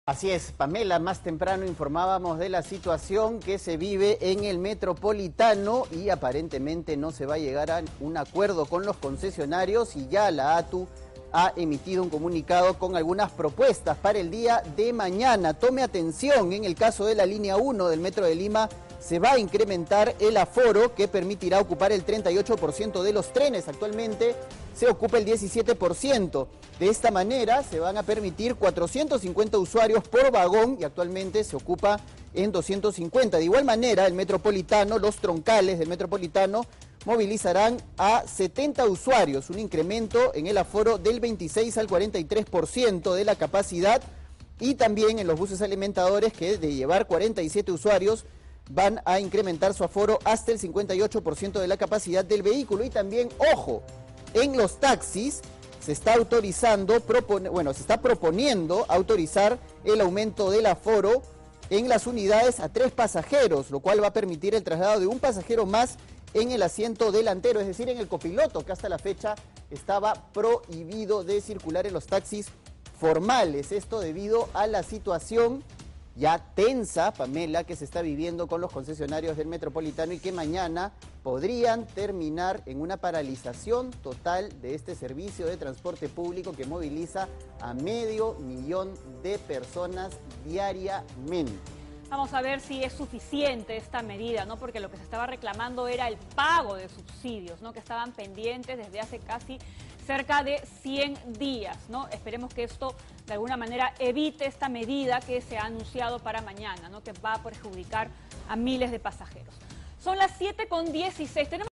ATU propone aumento de pasajeros en Metro de Lima, Metropolitano y taxis autorizados
Esta mañana, la Autoridad para el Transporte Urbano de Lima y Callao (ATU) propuso que los buses del Metropolitano, Metro de Lima y Callao y taxis autorizados, trasladen a una mayor cantidad de pasajeros para agilizar la espera en estaciones y paraderos.

Mediante un comunicado, la ATU señaló que la propuesta fue presentada en conjunto con el Ministerio de Transporte y Comunicaciones y que cuenta con la validación del Ministerio de Salud.

"Esta medida permitirá atender la creciente demanda en el transporte público formal, evitar aglomeraciones, colas y reducir los riesgos de contagio del COVID-19″, se lee en el pronunciamiento.

METRO DE LIMA

En el caso de la Línea 1, el incremento del aforo permitirá ocupar el 38% de los trenes. De esta manera de trasladará 450 usuarios aproximadamente en cada tren.

METROPOLITANO

Los buses troncales del Metropolitano movilizarían a 70 usuarios, lo que supone un incremento en el aforo de 26% a 43% de capacidad. Asimismo, los buses alimentadores podrán llevar 47 usuarios, es decir un aumento en su aforo actual hasta el 58% de la capacidad del vehículo.

TAXIS AUTORIZADOS

Los taxis autorizados se propone subir el aforo de las unidades a 3 pasajeros, lo cual permitirá trasladar a un pasajero más en el asiento delantero (copiloto).